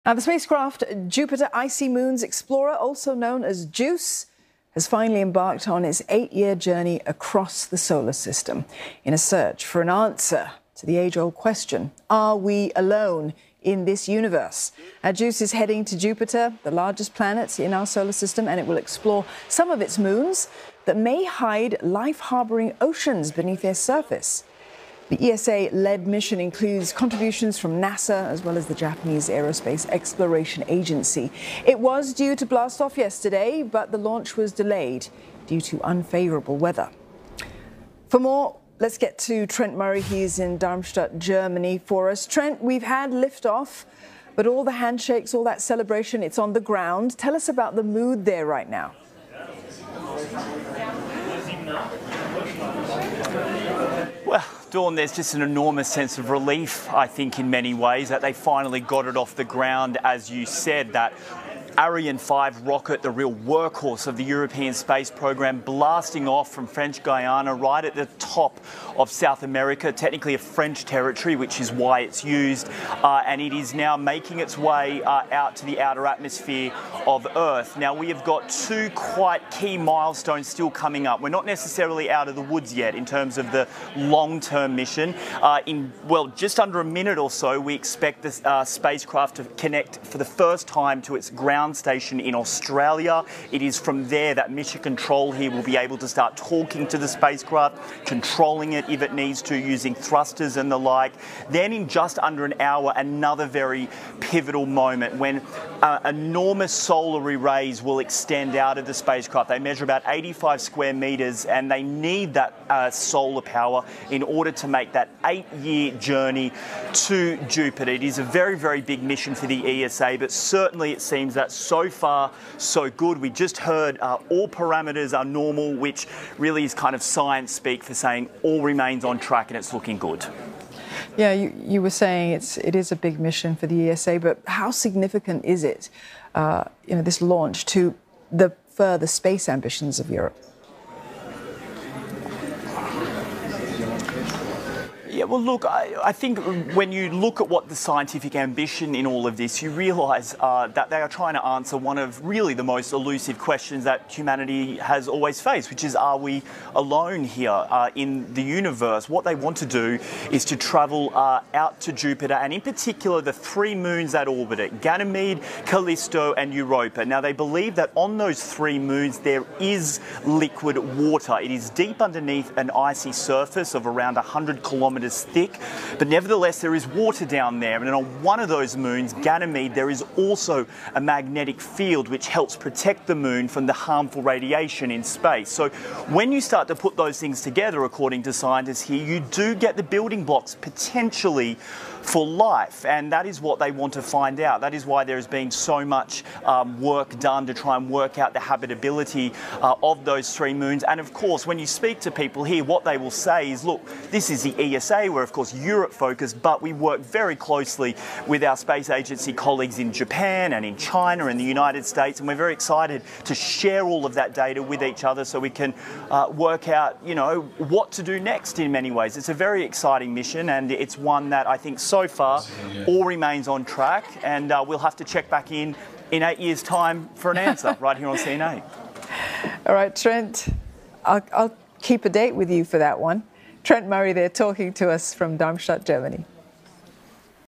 European Space Agency's JUICE orbiter to explore Jupiter's icy moons
The spacecraft Jupiter Icy Moons Explorer, also known as JUICE, has finally embarked on its eight-year journey across the solar system, in a search for an answer to the age-old question "are we alone in the universe?" JUICE is heading to Jupiter and will explore some of its moons that may hide life-harbouring oceans beneath their surface.